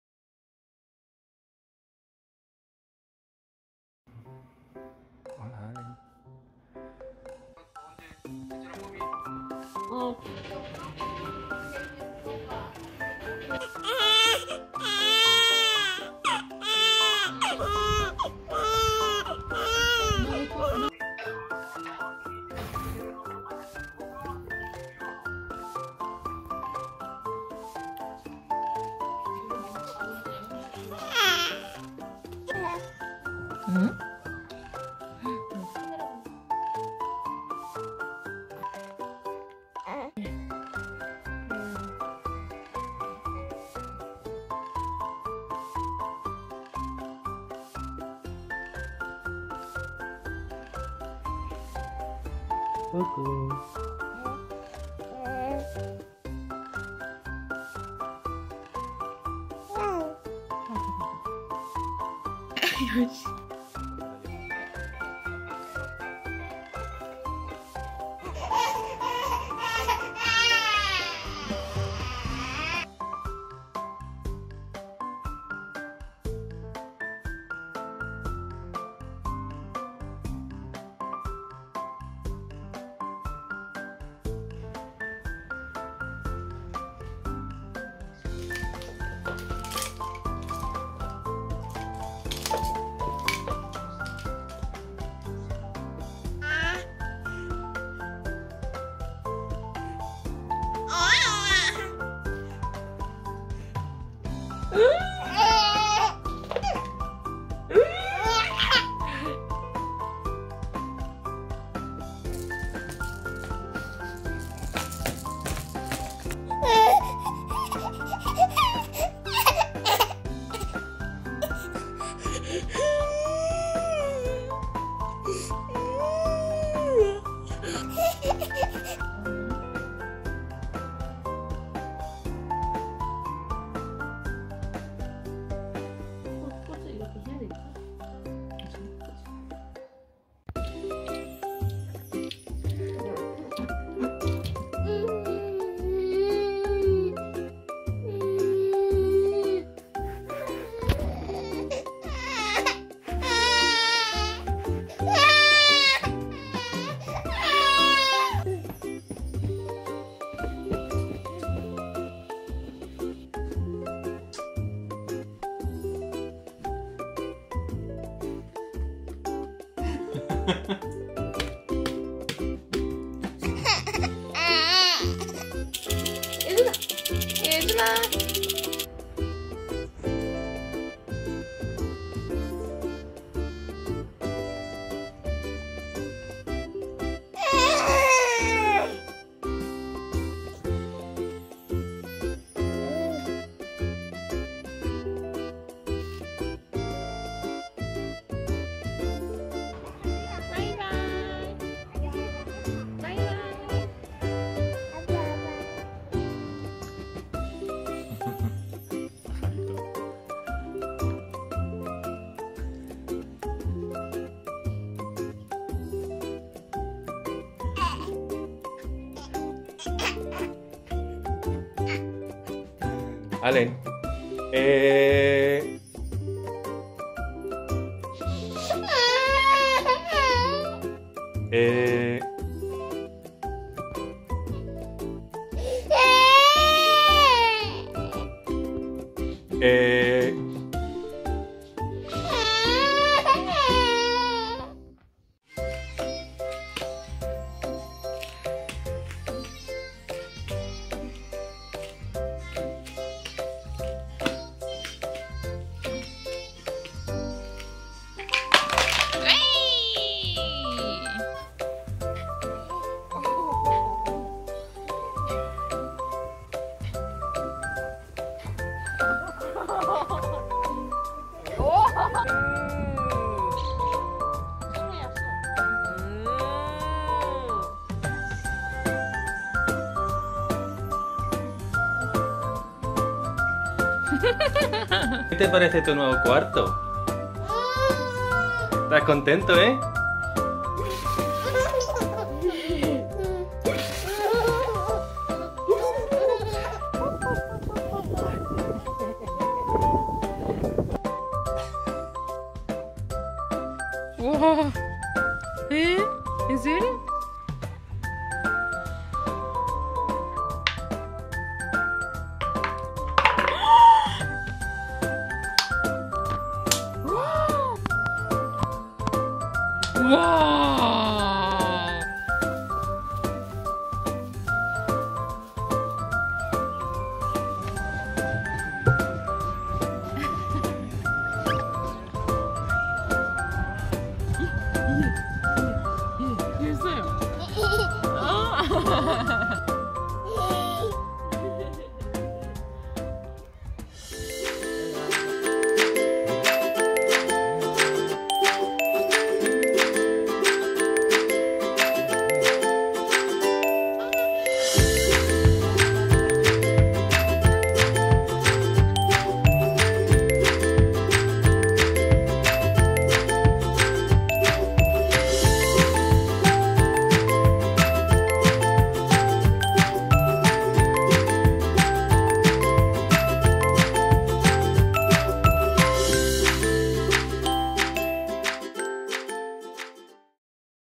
El primer año de nuestro bebé mes a mes
ESP
Hoy hemos celebrado el primer cumpleaños de nuestro bebé, y hemos preparado este pequeño video recopilatorio del primer año de Alen mes a mes. Ha sido un año duro pero a la vez muy especial y lleno de alegría que nos trae nuestro pequeño con cada nuevo paso que da.

ENG
Today we have celebrated our baby's first birthday, and we have prepared this little compilation video of Alen's first year month by month. It has been a hard year but at the same time very special and full of joy that our little one brings us with each new step he takes.